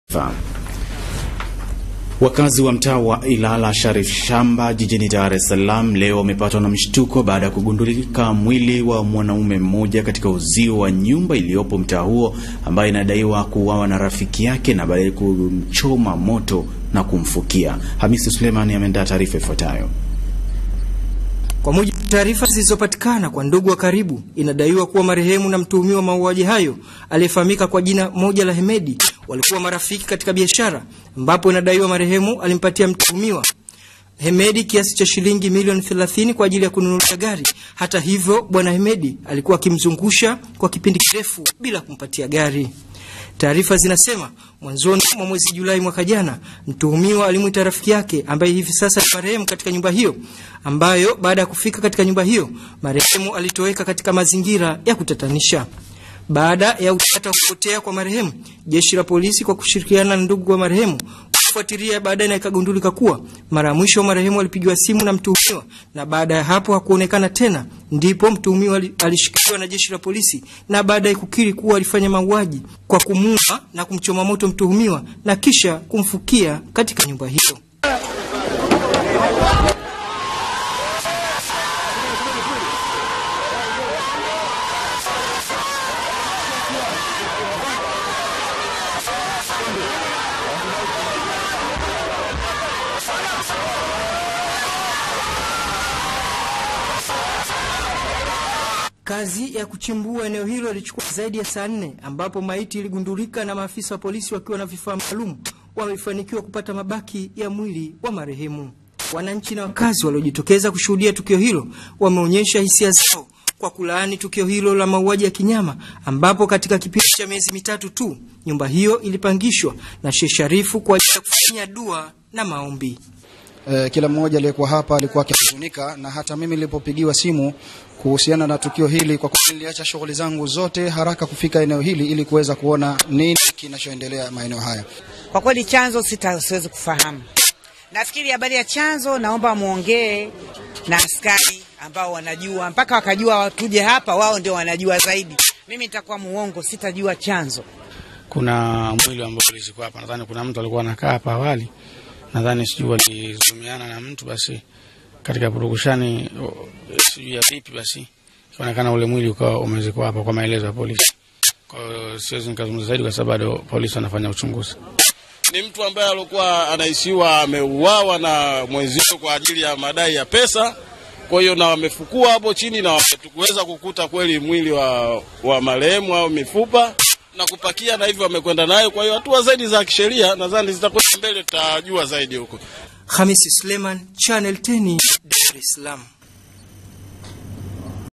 Amuua Rafiki Yake | CH10 HABARI
Mtu 1 jijini Dar es salaam anatuhumiwa kumuua rafiki yake,kumchoma moto na kisha kumfukia kwenye uzio wa nyumba kwa kinachodaiwa kuwa deni kati yao.